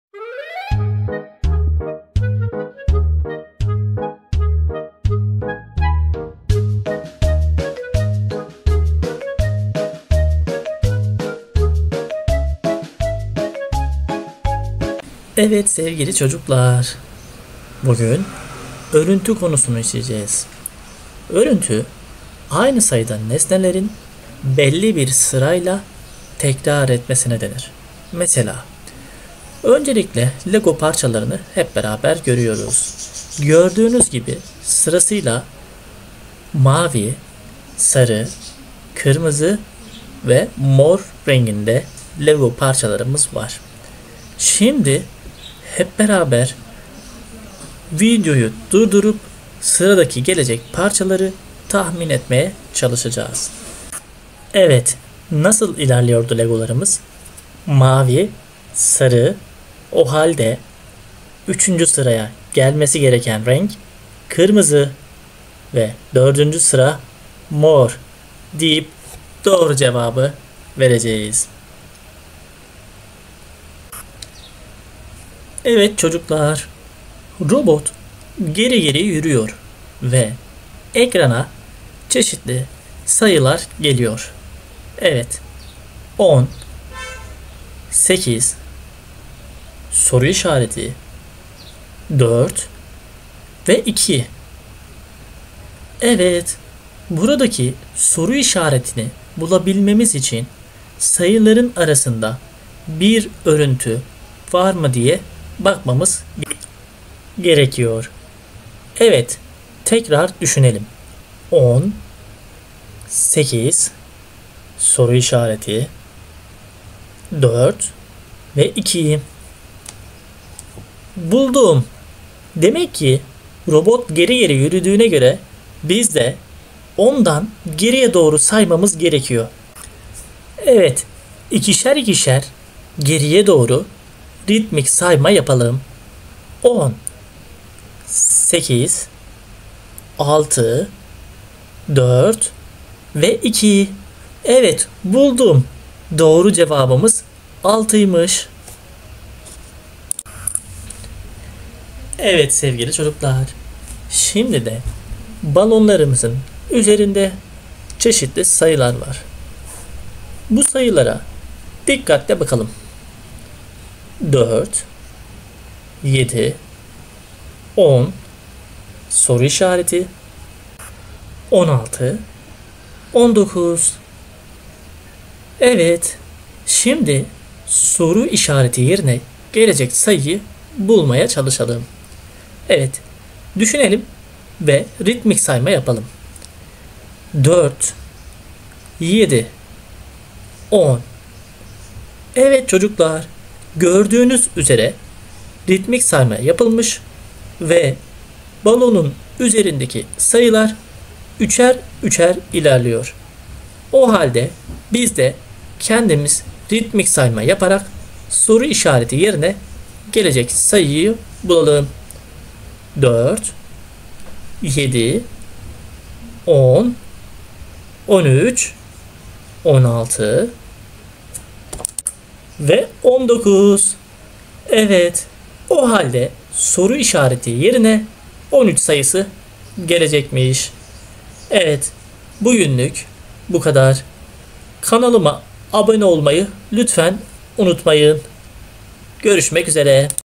1. Sınıf Matematik | Sayı ve Şekil Örüntüleri | Robot ve Gizli Sayı
Bu videoda, 1. sınıf matematik müfredatında yer alan örüntü konusunu eğlenceli ve kolay anlaşılır bir şekilde öğreniyoruz!

Videoda renkli nesneler veya figürler sırayla ekrana geliyor ve tekrarlayan düzenler oluşturuyor.
Çocuklar bu düzenleri fark ederek, örüntü kavramını keşfediyor ve devam ettirmeyi öğreniyor.

🎯 Kazanım:
Bu video, 2024 MEB Matematik Öğretim Programı kapsamında yer alan
KB2.4.1 – Nesnelerle örüntü oluşturur ve devam ettirir kazanımına uygundur.

🎓 Videoyu izleyerek öğrenciler:
✅ Görsel örüntüleri tanır ve fark eder
✅ Sıralı düzenleri takip eder
✅ Dikkat ve tahmin becerilerini geliştirir

🧠 Hadi birlikte izleyelim, sıradaki nesnenin ne olacağını tahmin edebilir misin?